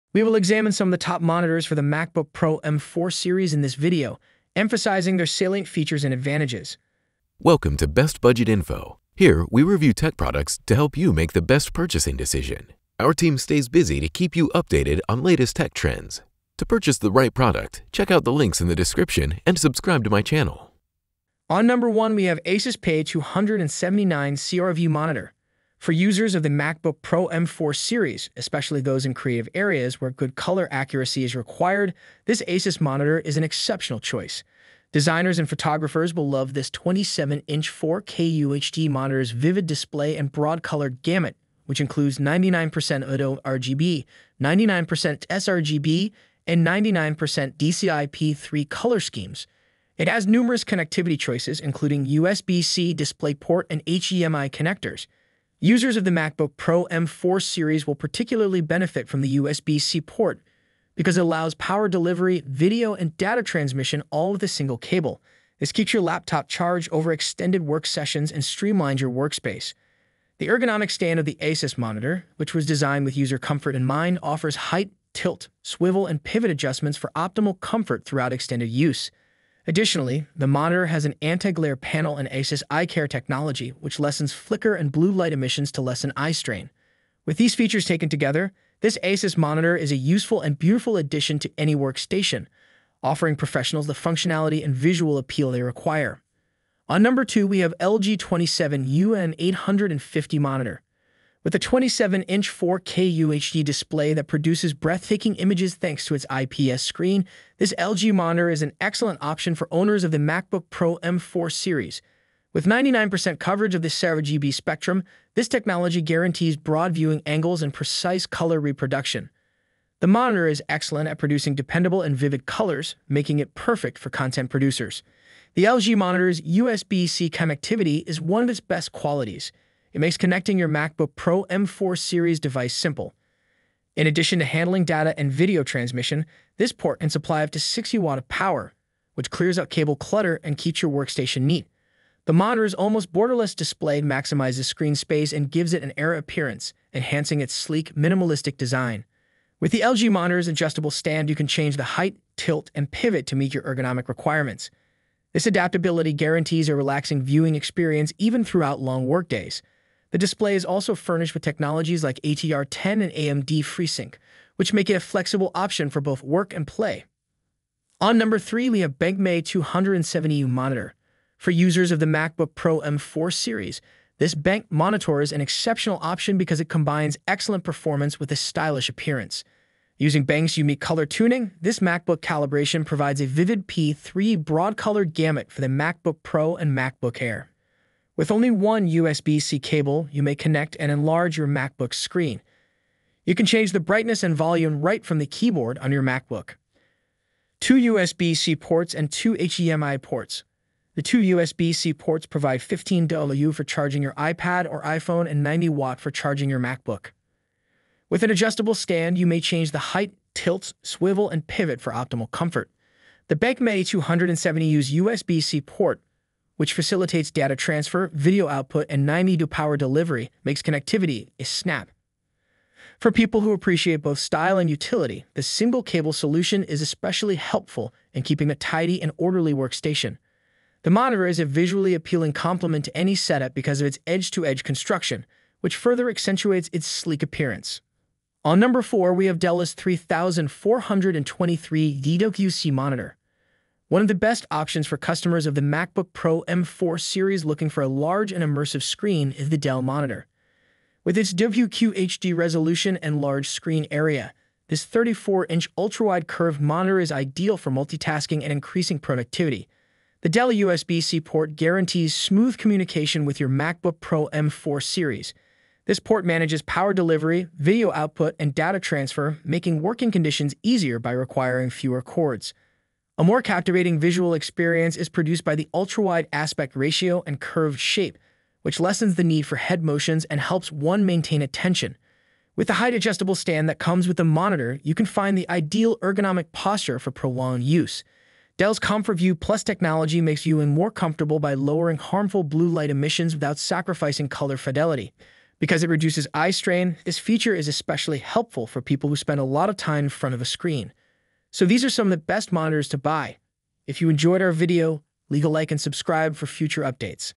Top External Monitors for Macbook Pro M4, M4 Pro, M4 Max
We will examine some of the top monitors for the Macbook Pro 16 and 14 M4, M4 Pro, M4 Max in this video, emphasizing their salient features and advantages.

1. Asus PA279CRV Monitor for MacBook Pro M4 series

2. LG 27UN850 Monitor for M4 series MacBook Pro

3. Benq MA270U Macbook pro M4 Series monitor

4. Dell S3423DWC Monitor for MacBook Pro M4 series

More Monitors